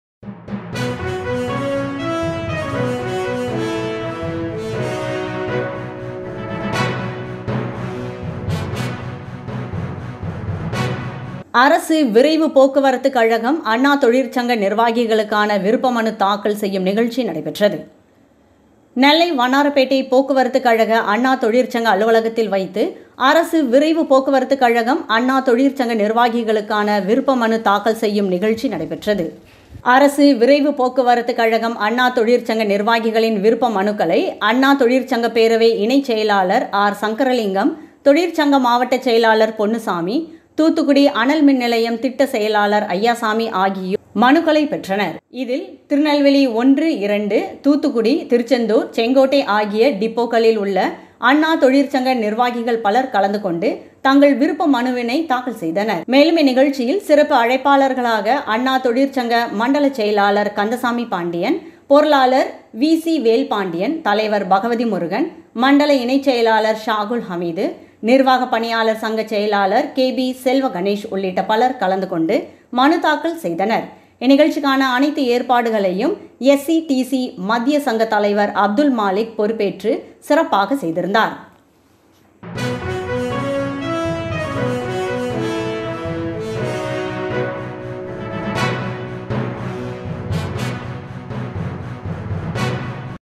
அரசு விரைவு போக்குவரத்து கழகம் அண்ணா தொழிற்சங்க நிர்வாகிகளுக்கான விருப்ப மனு தாக்கல் நிகழ்வு | TAMIL
நெல்லை வண்ணார்பேட்டை போக்குவரத்து கழக அண்ணா தொழிற்சங்க அலுவலகத்தில் வைத்து நடைபெற்றது    அரசு விரைவு போக்குவரத்து கழகம் அண்ணா தொழிற்சங்க நிர்வாகிகளின் விருப்ப மனுக்களை அண்ணா தொழிற்சங்கப் பேரவை இணைச் செயலாளர் ஆர் சங்கரலிங்கம் தொழிற்சங்க மாவட்ட செயலாளர் பொன்னுச்சாமி தூத்துக்குடி அனல் மின் நிலையம் திட்ட செயலாளர் அய்யாசாமி ஆகியோரிடம்  மனு தாக்கல் செய்தனர் இதில் திருநெல்வேலி 1, 2.தூத்துக்குடி திருச்செந்தூர் செங்கோட்டை ஆகிய டிப்போ களில் உள்ள அண்ணா தொழிற்சங்க நிர்வாகிகள்பலர் கலந்து கொண்டு  தங்கள் விருப்ப மனுவினை தாக்கள் செய்தனர் மேலும் நிகழ்வில் சிறப்பு அழைப்பாளர்களாக அண்ணா தொழிற்சங்க மண்டல செயலாளர் கந்தசாமி பாண்டியன் பொருளாளர் வி. V.C.வேல் பாண்டியன், தலைவர் பகவதி முருகன் ,மண்டல இணைச் செயலாளர் சாகுல் ஹமீது நிர்வாக பணியாளர் சங்க செயலாளர் கே.பி. செல்வ கணேஷ் உள்ளிட்ட பலர் கலந்து கொண்டு மனு தாக்கல் செய்தனர் இந்நிகழ்ச்சிக்கான அனைத்து ஏற்பாடுகளையும்SETC மத்திய சங்க தலைவர் அப்துல் மாலிக் பொறுப்பேற்று வெகு சிறப்பாக செய்திருந்தார்